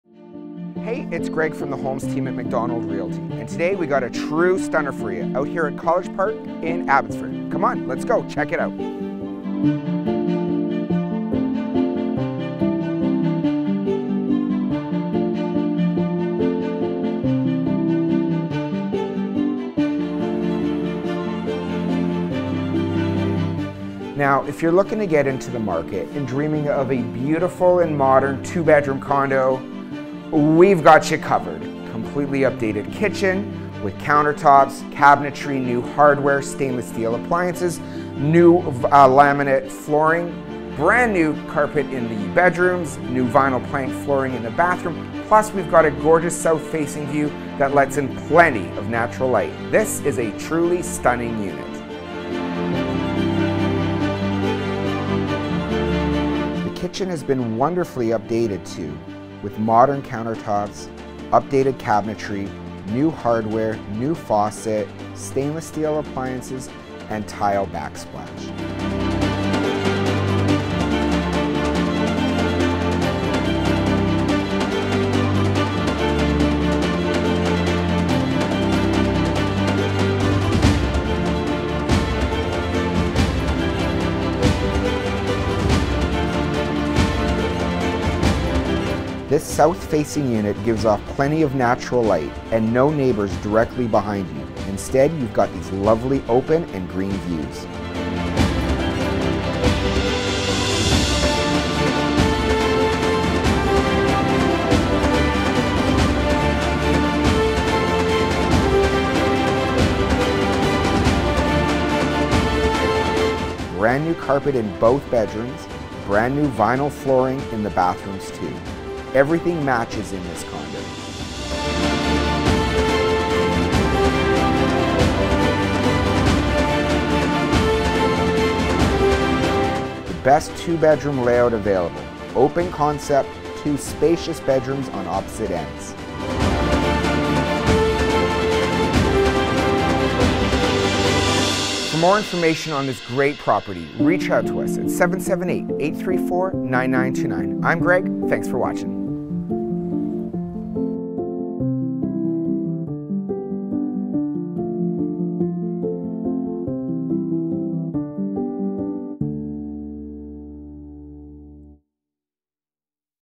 SOLD OVER ASKING! 306-33668 King Road, Abbotsford - The Holmes Real Estate Team
Beautiful 2-bed, 2-bath, 926 sq ft, south-facing condo at College Park. You'll love the modern colours and wonderful
updates this home provides. Gorgeous laminate flooring, updated kitchen with undermount sink, new faucet & S/S
appliances. Brand new carpets in both bedrooms. New vinyl flooring in the bathrooms & laundry. Every matches in
this home!! Terrific floorplan with bedrooms opposite each other. South facing views overlooking the greenery from
the new complex next door. Conveniently located just off Hwy 1 and close to shopping and UFV. No age restrictions
either. Hurry on this pretty unit - it won't last!